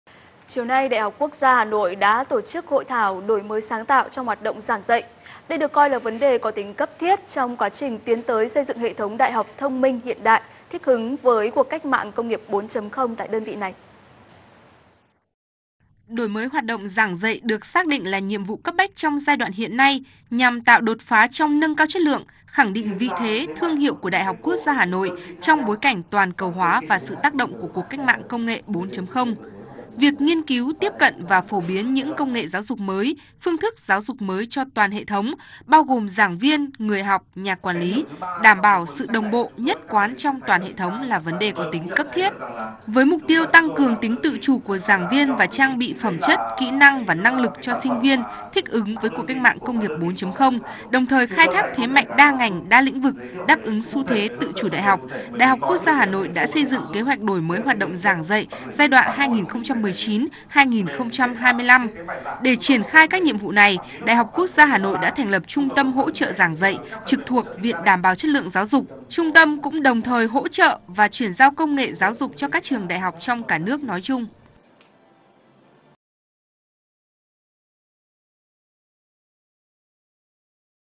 Đổi mới giảng dạy đại học trong thời kỳ 4.0
VTC1 | Đổi mới sáng tạo trong hoạt động giảng dạy là một trong những vấn đề cấp thiết trong quá trình tiến tới xây dựng hệ thống đại học thông minh, hiện đại, thích ứng với cuộc cách mạng công nghiệp 4.0